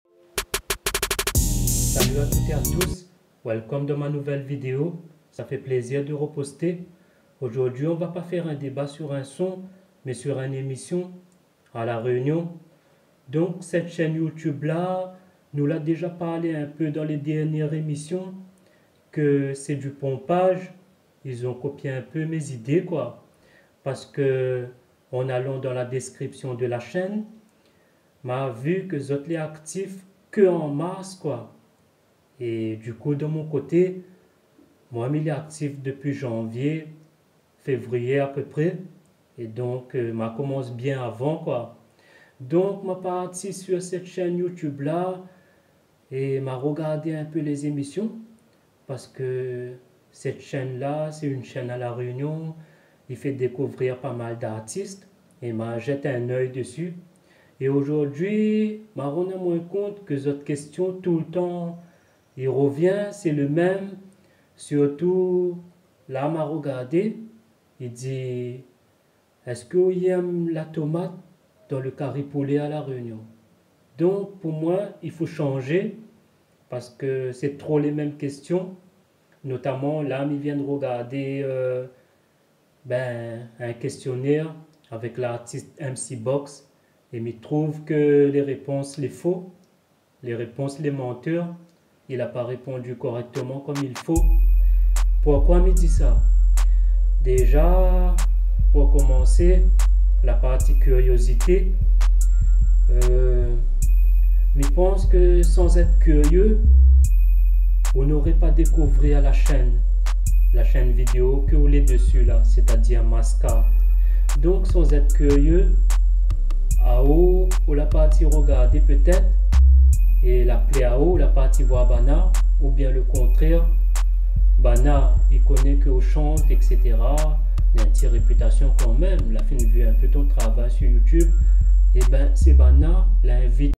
eroskiller ne valide pas l'émission Mc Box - Ou valide ? Ou valide pas ?
rap story